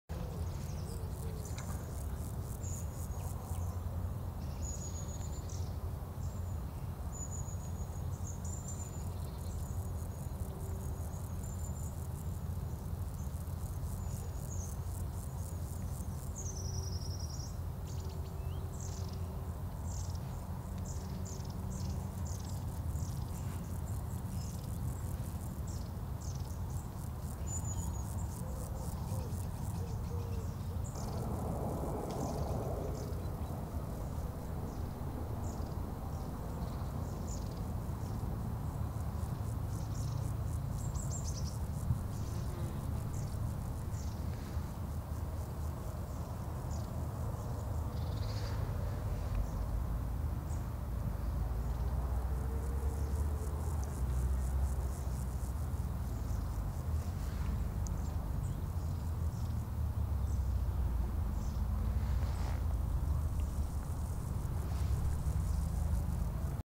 Nymph of Green Shield bug sucks sap from flower bud 🔴
Description (more): A nymph of the Green Shield bug sucks juice from a flower bud of the musk Mallow. The Nymph is angled from the front. The muzzle is well visible. Composite clip.